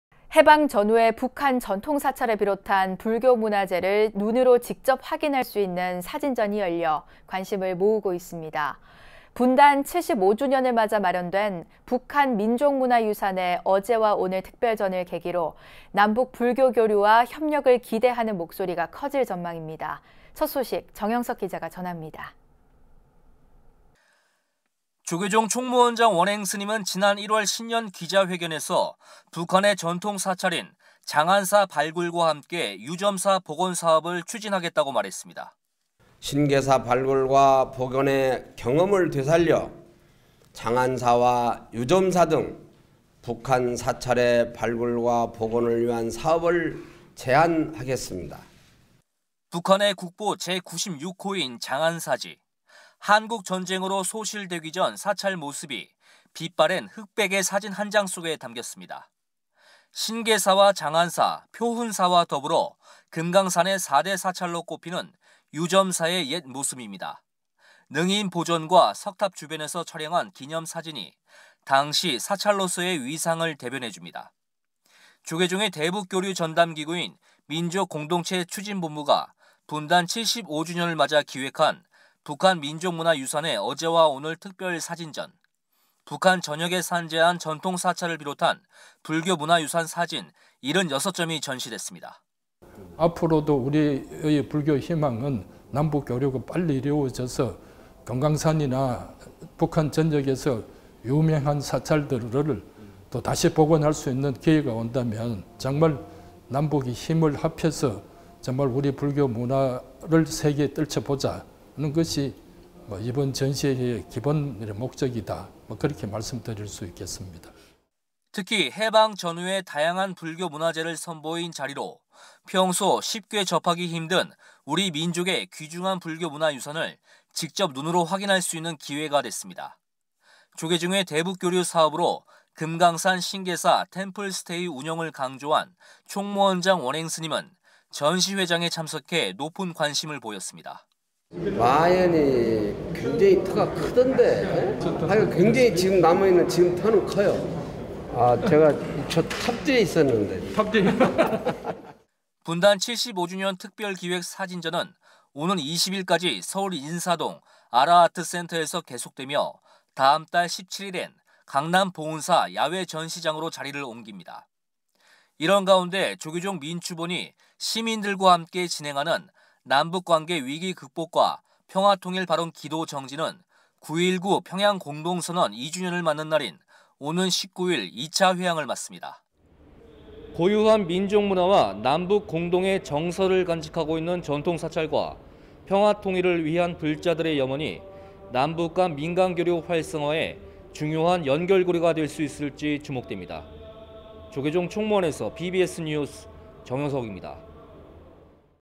[BBS뉴스] "남북은 하나"…사진으로 만나는 북한 불교문화유산
해방 전후의 북한 전통사찰을 비롯한 불교 문화재를 눈으로 직접 확인할 수 있는 사진전이 열려 관심을 모으고 있습니다.

분단 75주년을 맞아 마련된 '북한 민족문화유산의 어제와 오늘' 특별전을 계기로 남북 불교 교류와 협력을 기대하는 목소리가 커질 전망입니다. 첫 소식 정영석 기자가 전합니다.